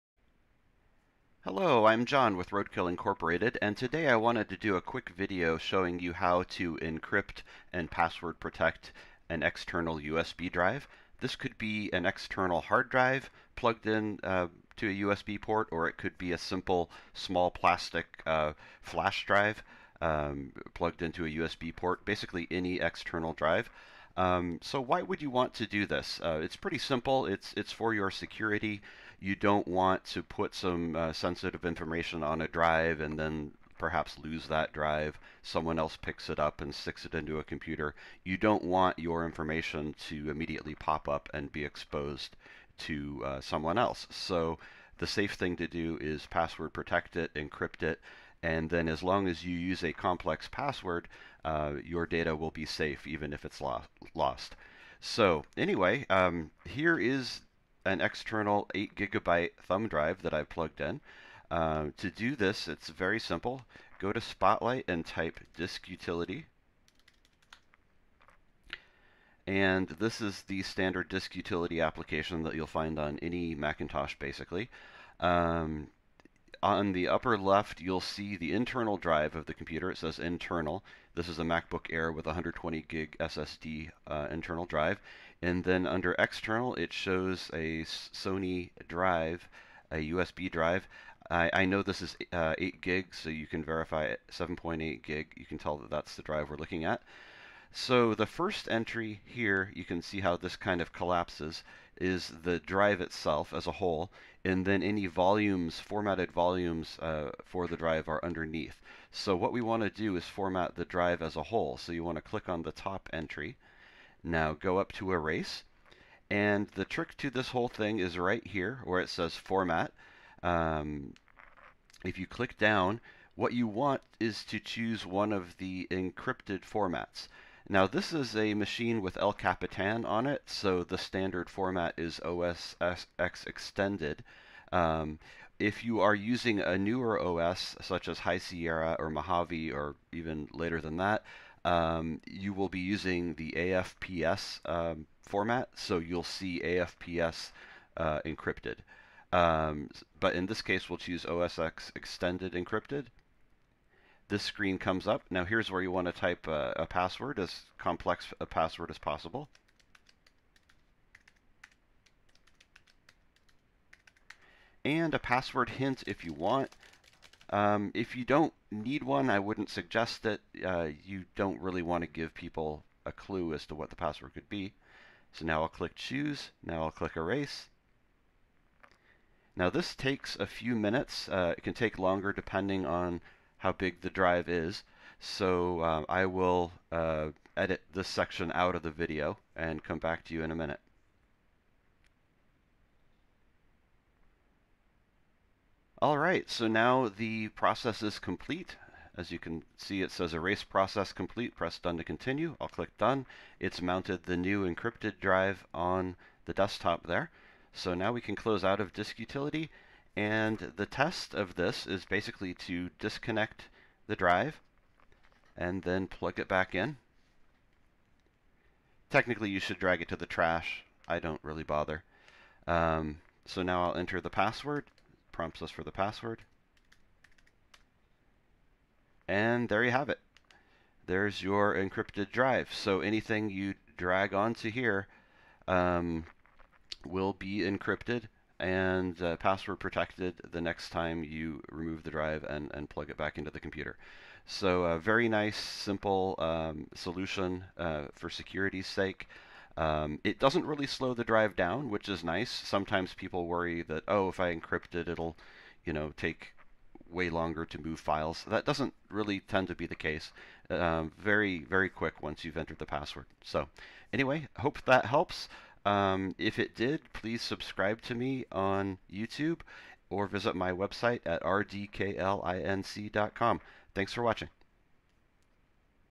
Encrypting and Password Protecting a USB Flash Drive on a Mac
Demonstrates the simple process of encrypting and password protecting a USB drive on a Mac.  It takes just a few minutes and gives you a 100% secure means of storing your information.